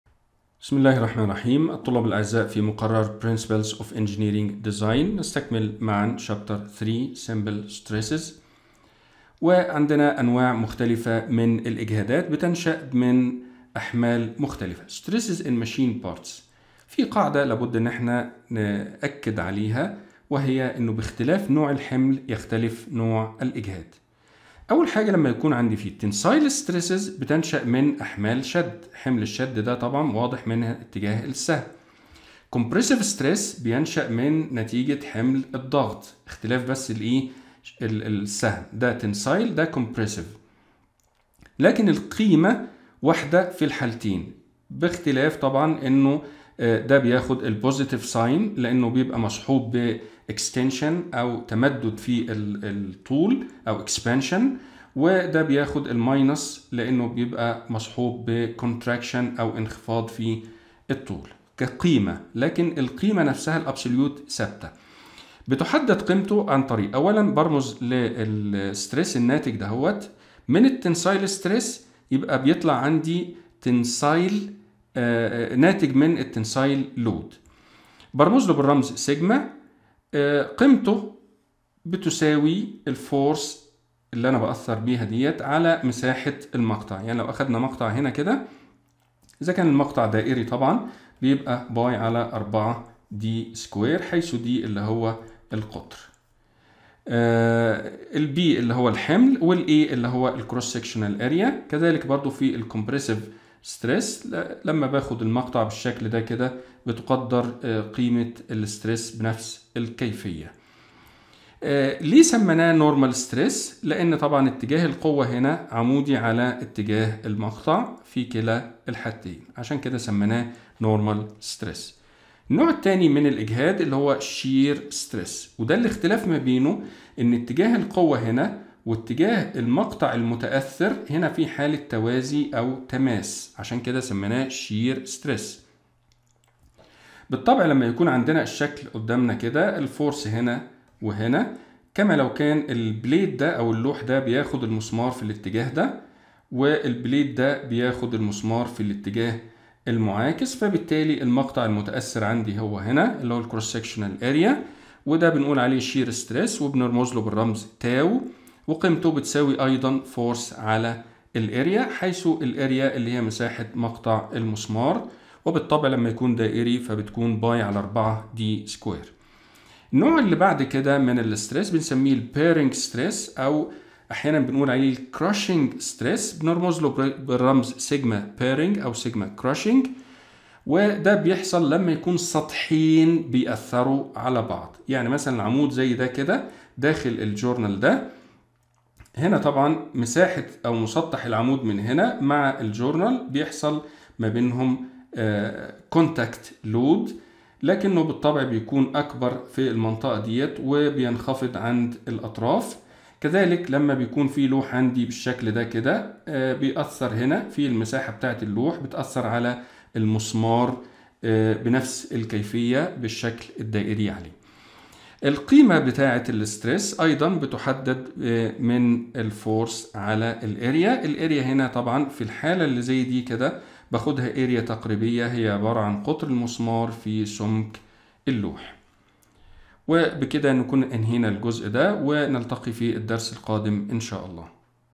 Principles of Engineering Design | 3-2 | Stresses in Machines - 1
الوحدة 3 : Simple Stresses in Machine Parts
الدرس 2 : Stresses in Machines - 1
- Tensile and Compressive stresses.
- Shearing Stress.
- Bearing Stress.